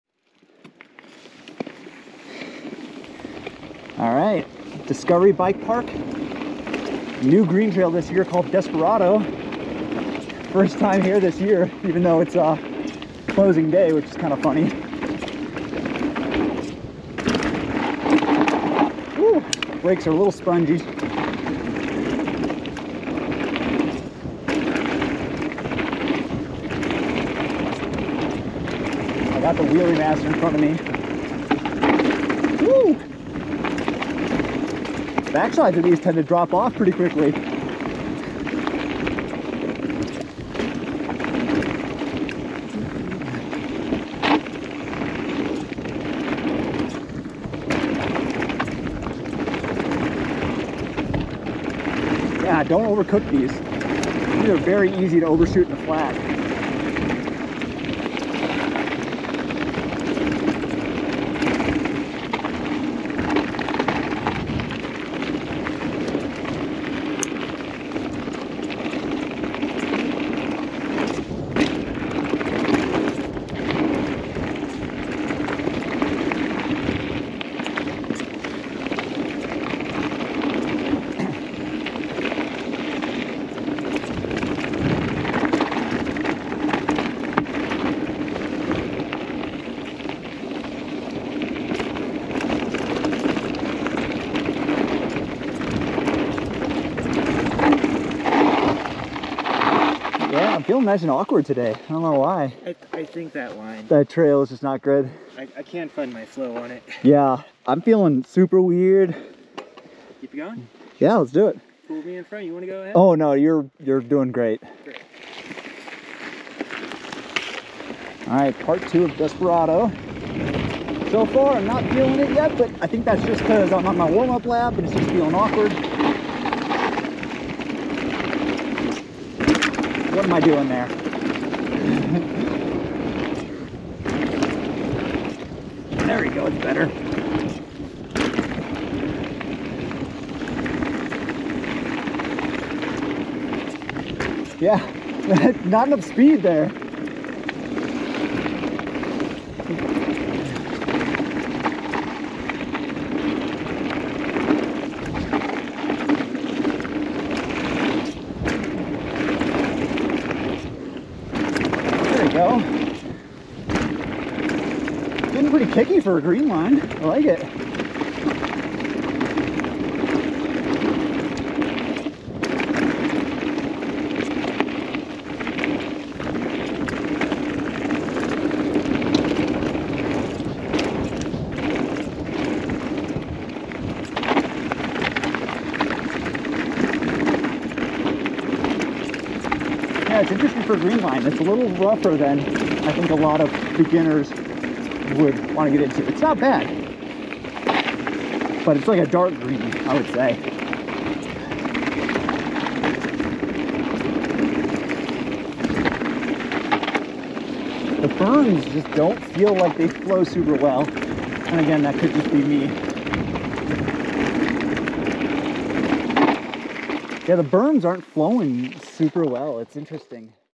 Desperado - Discovery Bike Park - Phillipsburg, MT
Desperado at Discovery Bike Park in Phillipsburg, MT
Stats: →0.9mi   ↑2ft   ↓586ft
October 15, 2023

0:00 Upper Desperado
1:45 Lower Desperado